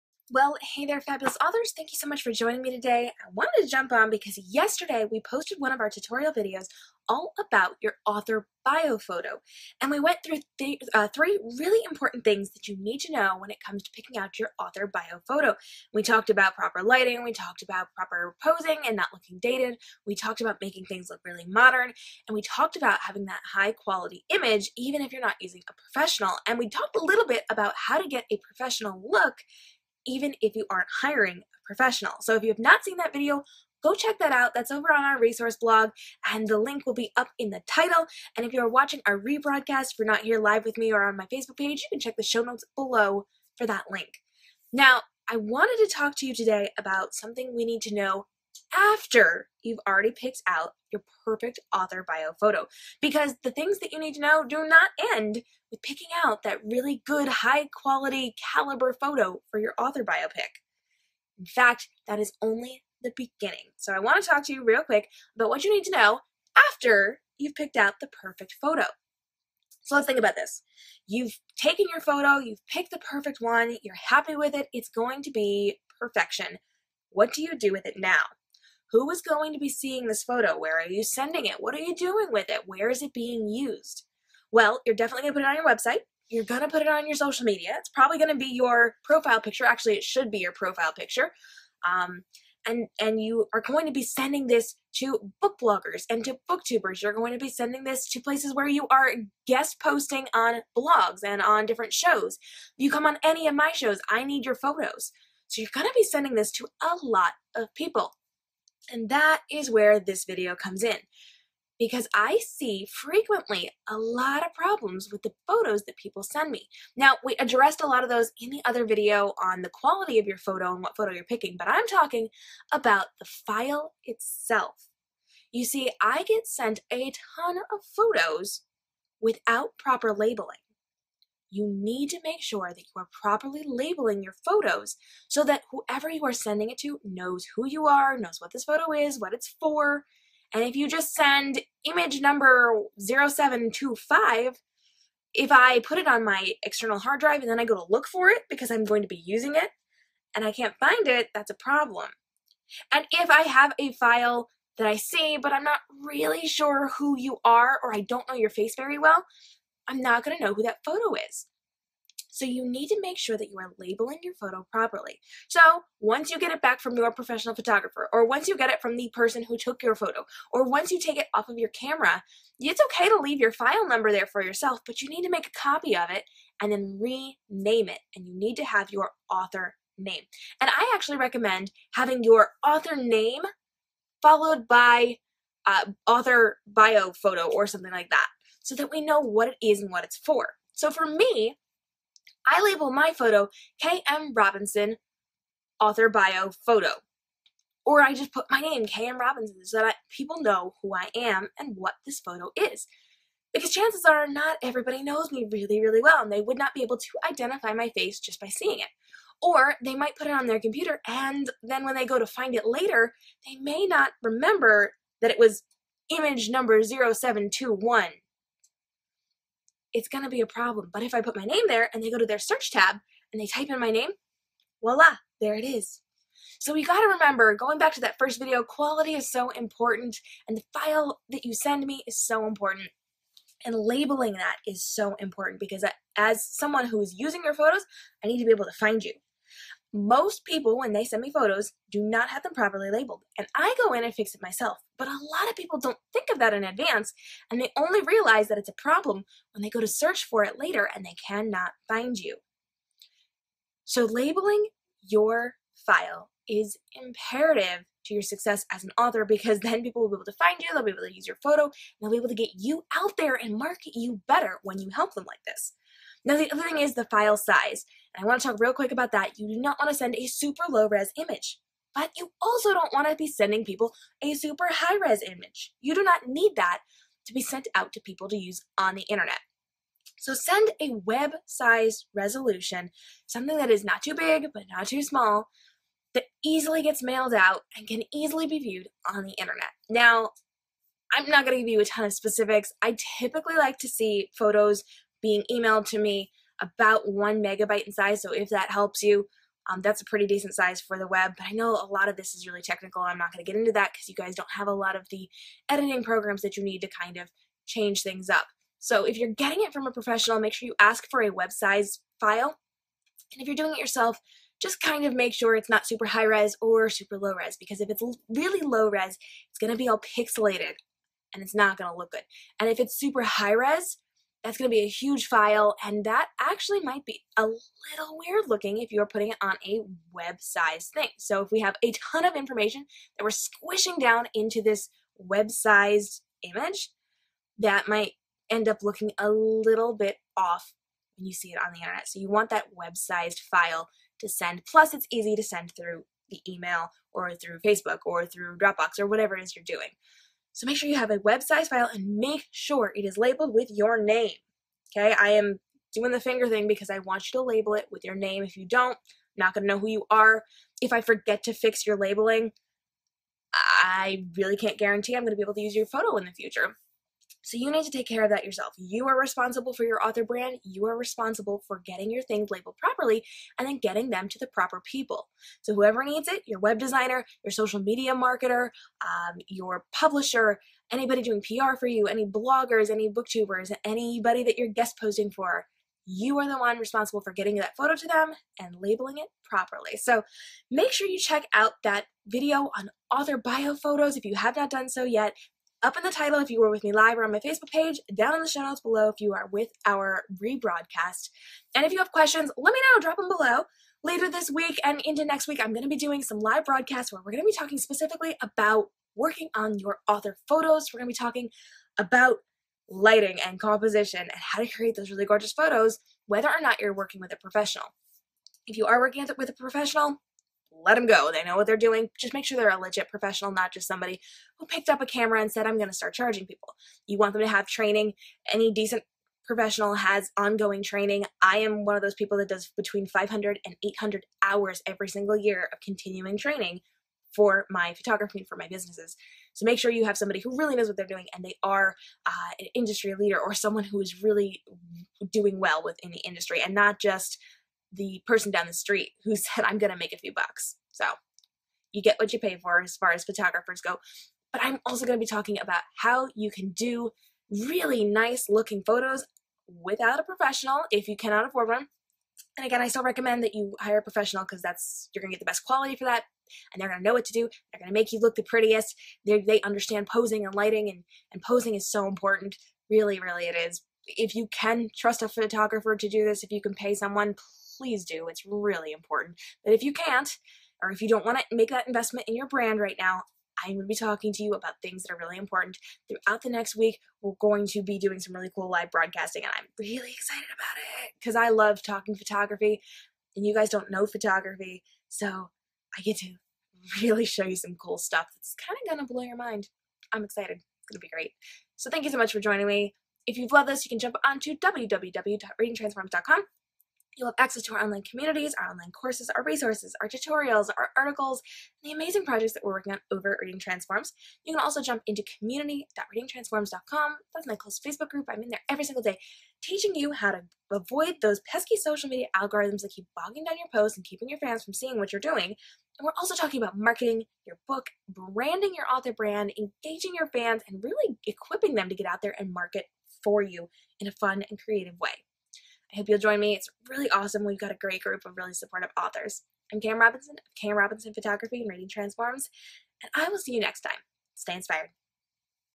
Author Bio Photos: What You Need To Know AFTER You Pick Your Photo
After you've picked out your author photo, naming and sizing your files correctly are essential.